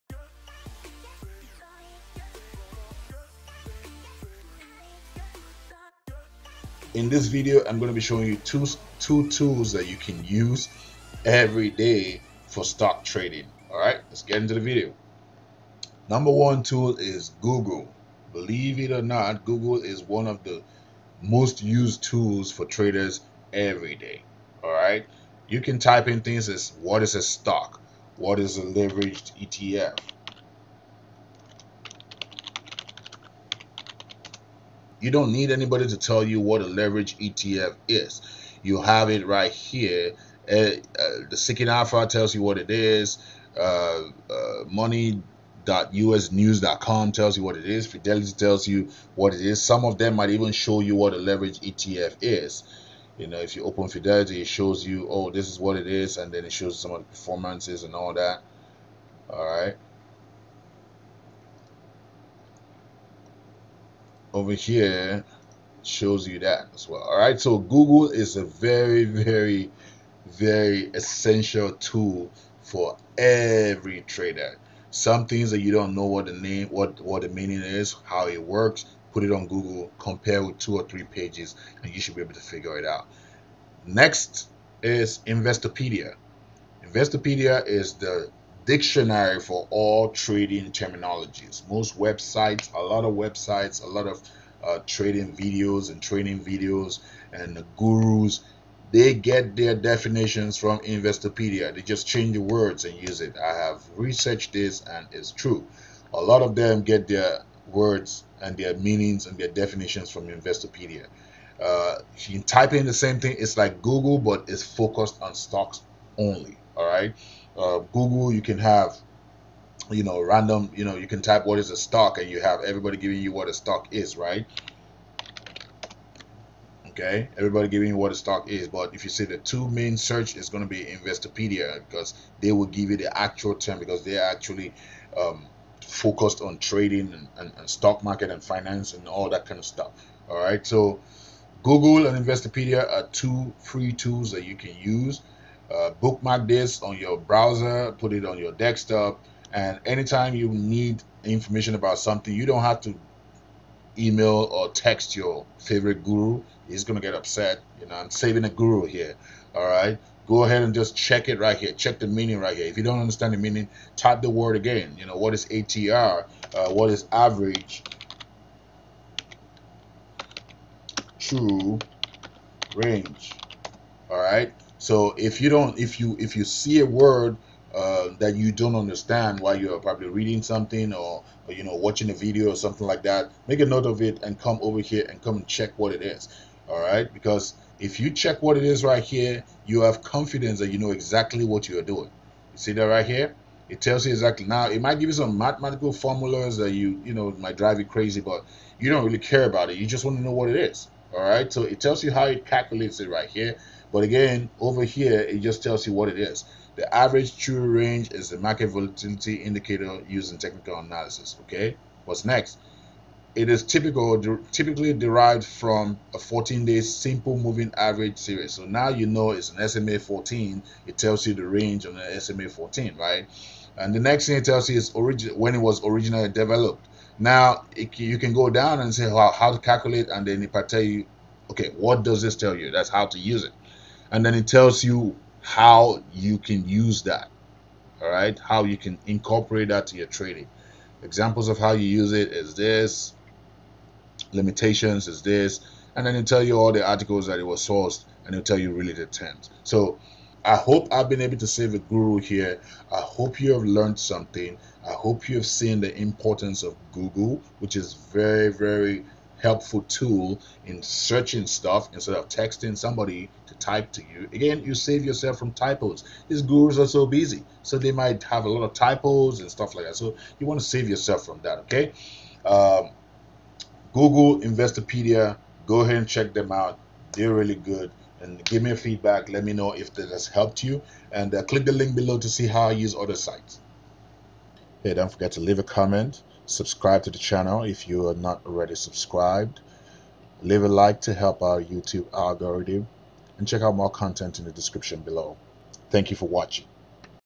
Video Lesson #280: How to trade stocks using google (Beginner)
1. Free Tips
2. How to trade stocks using google
3. How to find trade ideas using google

Process and preparation always meets opportunity